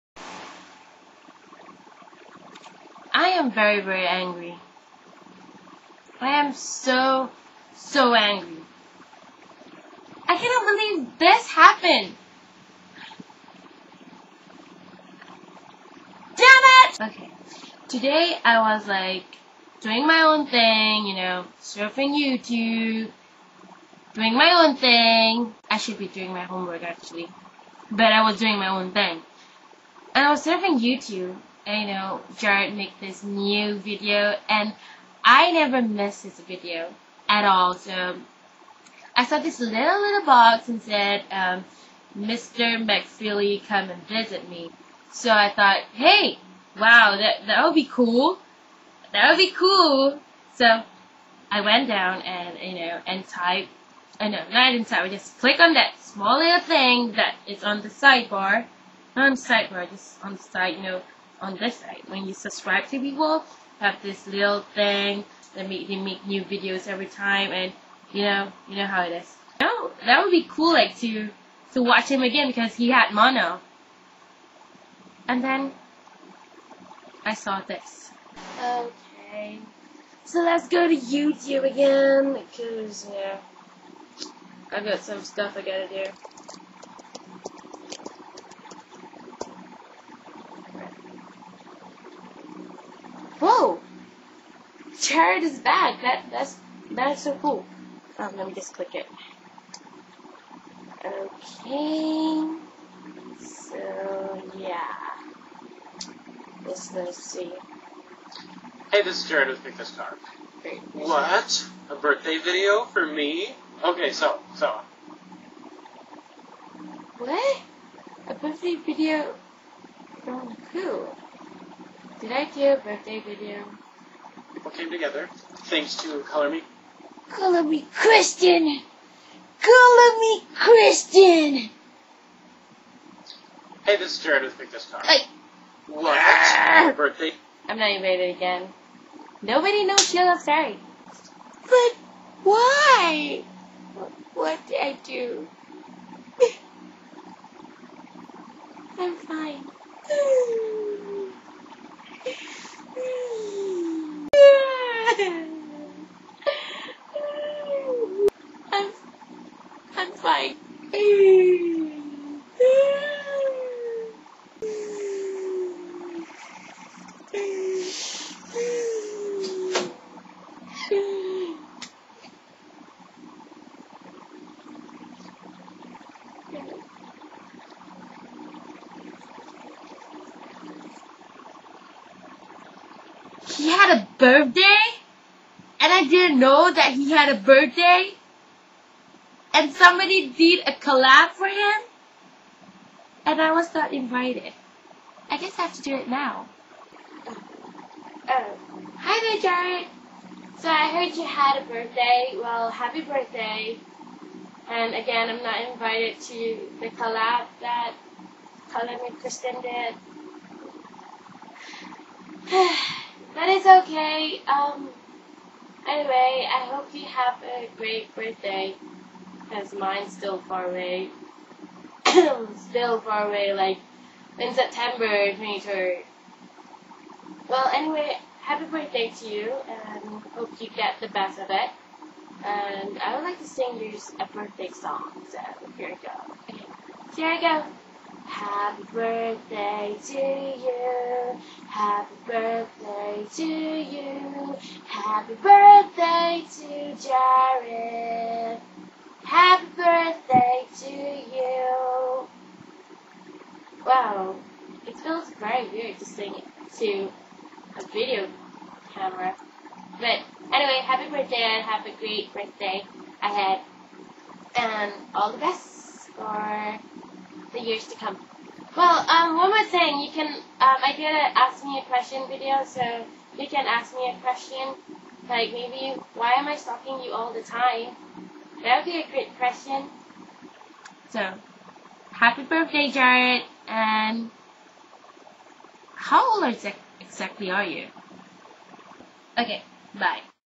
Whoa...not invited again? But HAPPY B'DAY JARED!
Happy birthday Jared. Here's my video for you. since I didn't know about the collab. But it's ok!